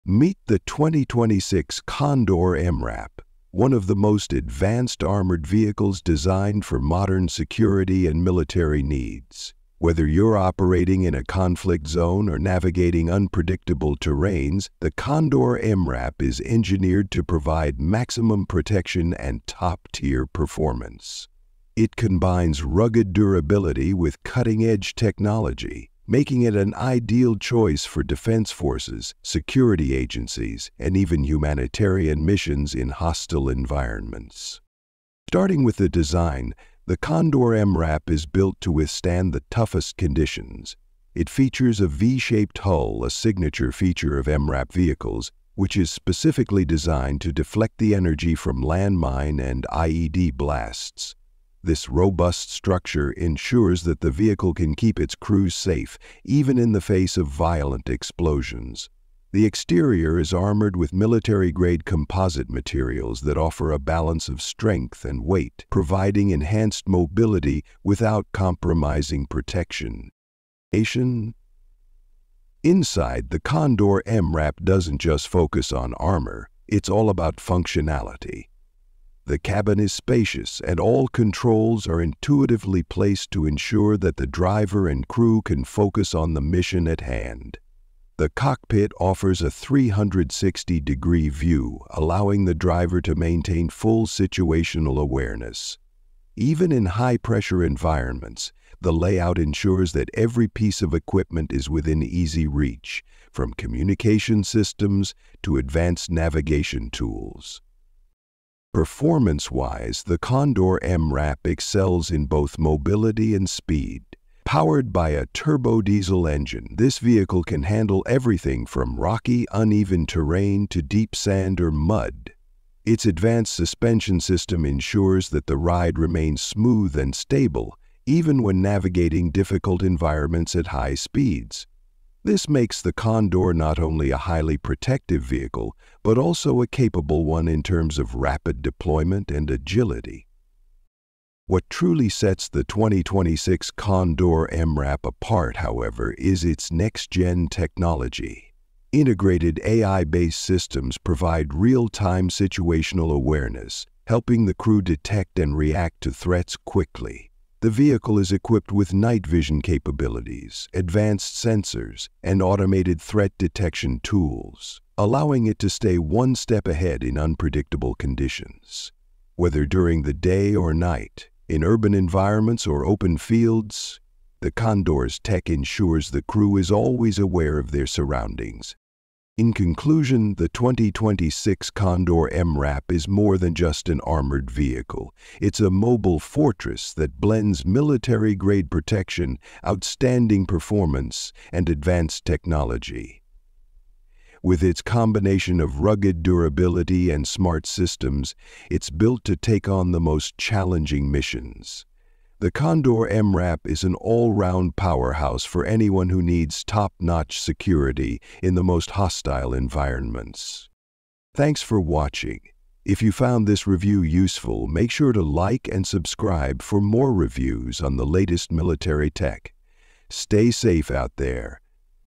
2026 Candor MRAP: Ultimate Armored Protection and Mobility
The 2026 Candor MRAP (Mine-Resistant Ambush Protected) vehicle is engineered for superior defense and tactical performance. Built to withstand the most challenging environments, it features advanced armor, a high-clearance chassis, and cutting-edge mobility systems. Equipped with powerful drivetrain technology and enhanced occupant safety measures, the Candor MRAP is ideal for military, security, and peacekeeping missions. Designed for reliability and versatility, it ensures unmatched protection and operational efficiency in the field.